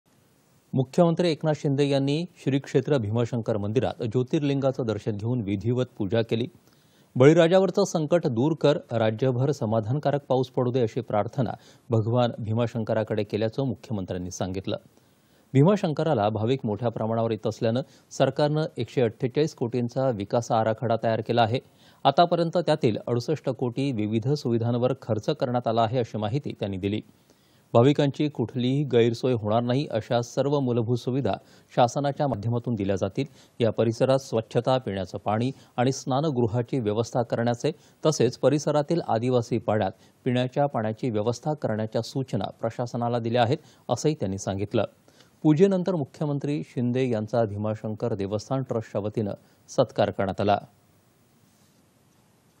बळीराजावरचं संकट दूर कर राज्यभर समाधानकारक पाऊस पडू दे अशी प्रार्थना - मुख्यमंत्री एकनाथ शिंदे
दि.११.०९.२०२३ :

    मुख्यमंत्री एकनाथ शिंदे यांनी श्री क्षेत्र भीमाशंकर मंदिरात ज्योतिर्लिंगाचं  दर्शन घेऊन विधिवत पूजा केली. बळीराजावरचं  संकट दूर कर, राज्यभर समाधानकारक पाऊस पडू दे, अशी प्रार्थना भगवान भीमाशंकराकडे केल्याचं,  मुख्यमंत्र्यांनी सांगितलं. भीमाशंकरला भाविक  मोठ्या प्रमाणावर येत  असल्यानं सरकारनं  १४८ कोटींचा विकास आराखडा तयार केला आहे. आतापर्यंत त्यातील ६८ कोटी विविध सुविधांवर खर्च करण्यात आला आहे,अशी माहिती त्यांनी दिली.  भाविकांची कुठलीही गैरसोय होणार नाही अशा सर्व मूलभूत सुविधा शासनाच्या माध्यमातून दिल्या जातील. या परिसरात स्वच्छता, पिण्याचं  पाणी आणि स्नानगृहाची व्यवस्था करण्याचे तसेच परिसरातील आदिवासी पाड्यात पिण्याच्या पाण्याची व्यवस्था करण्याच्या सूचना प्रशासनाला दिल्या आहेत, असंही त्यांनी सांगितलं. पुजेनंतर मुख्यमंत्री  शिंदे यांचा भीमाशंकर देवस्थान ट्रस्टच्या वतीनं  सत्कार करण्यात आला.

8.30am | 11.00am | 1.00pm | 4.30pm | 7.00pm | 9.30pm